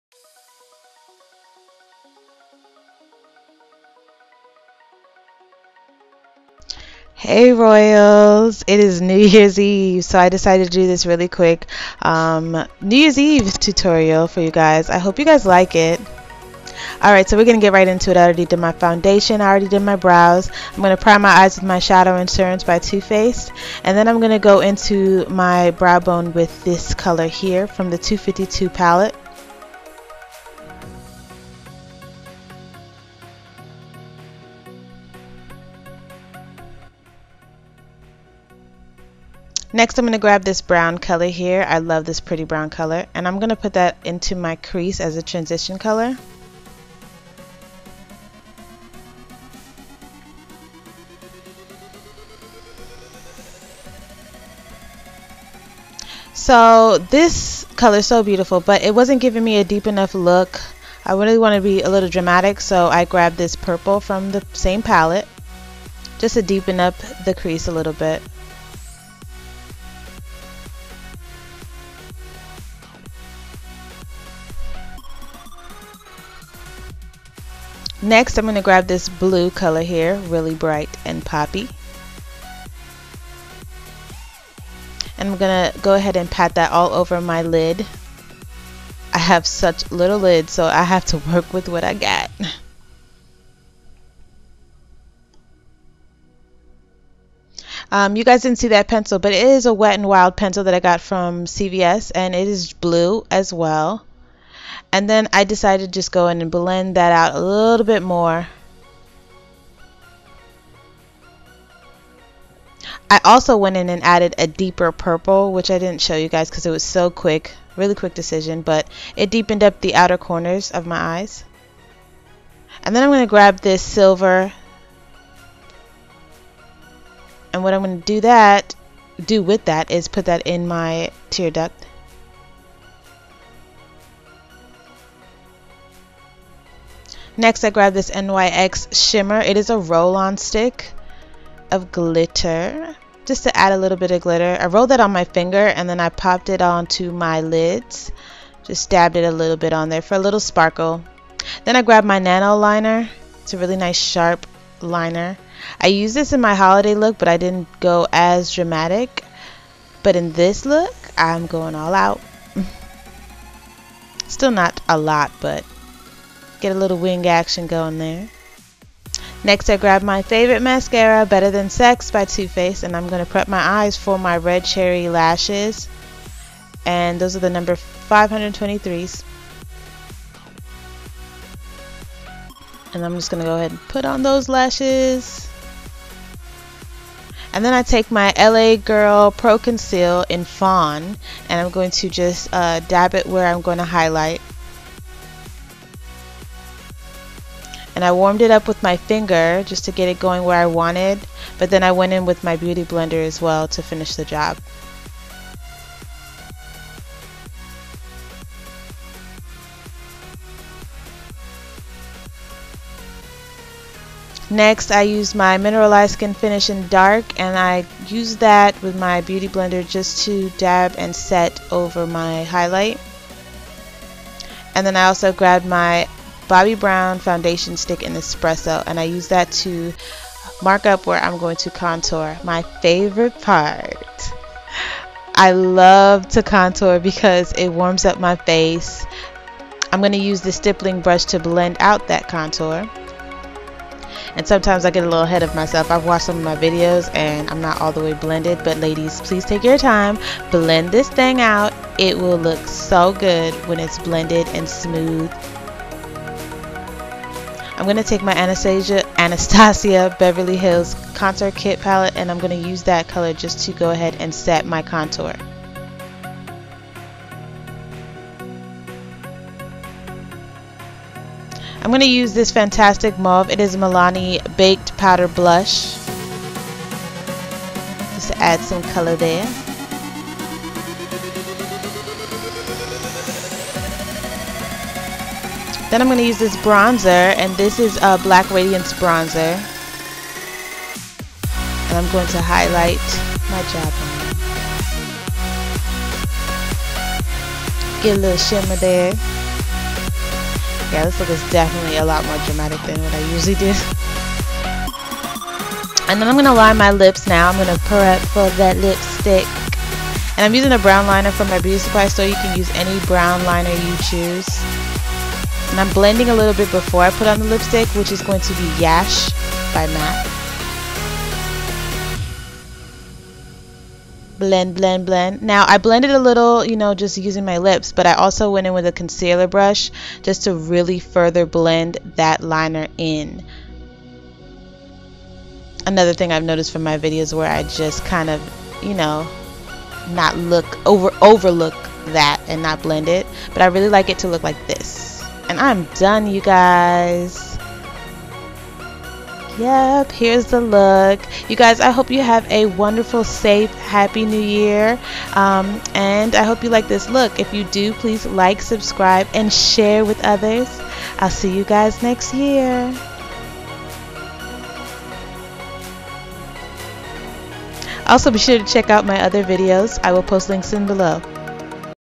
"STARRY NIGHT" NEW YEARS EVE MAKE UP LOOK
HAPPY NEW YEAR LOVES!

I'm uber late but decided to post this subtle w/ a hint of drama make up look.  I hope you like it and share share share! lol

PRODUCTS USED:

Too faced Cosmetics Primmed Poreless primer

Mac Studio Fix Fluid NW50

Anastasia BH dip brow "chocolate"

Mac Mineralized Skin Finish "Dark"

Bobby Brown foundation stick "espresso"

Anastasia Beverly Hills countour kit

252 Coastal Scents Pallette

Blue Wet n Wild eye pencil

Better than sex mascara

Absolute NANO liner

Milani Blush "Fantastic Mauve"

Black Radiance "gingersnap"

Mac's "Yash" lipstick